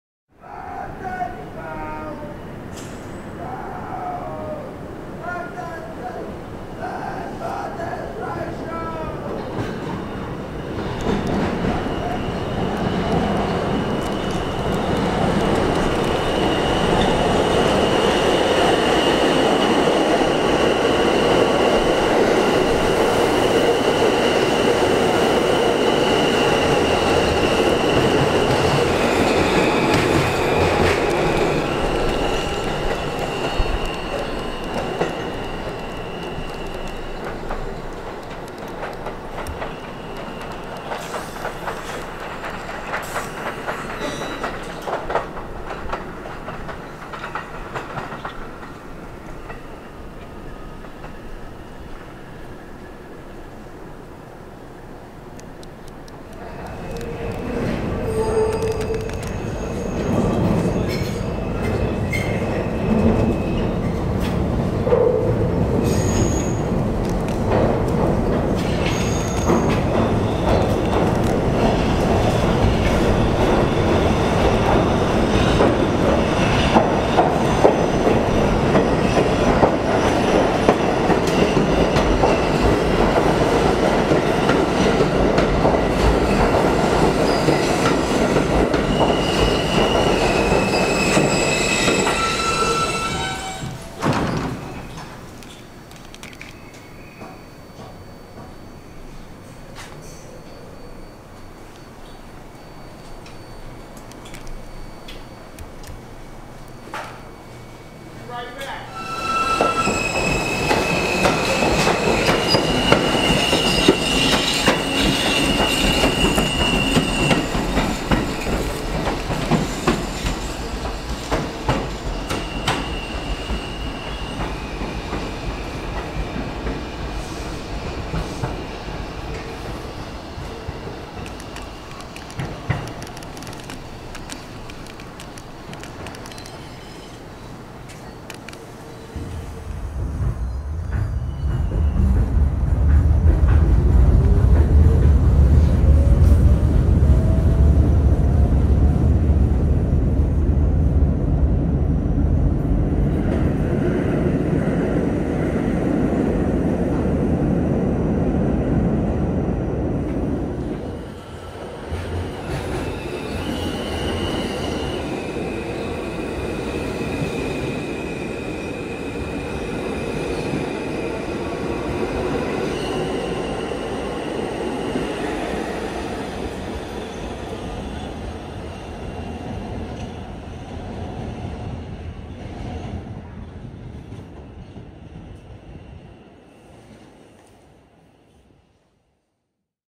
R160 Train on Franklin Avenue Shuttle (Park Place, "Malbone Street Wreck" Curve, Botanic Garden)
Here are several videos taken during the midnight hours of February 25, 2023, featuring the MTA NYC Subway - in particular, the Franklin Avenue Shuttle Line, which is notable for having only one bidirectional track at some spots.
During midnight hours, a 4-car train of Alstom R160A cars ran up and down the tracks of the Franklin Avenue Shuttle/Line to test out signals on the line. It is not common to see other railcars besides the two-car R68 trains used on Franklin Avenue Shuttle trains on this line!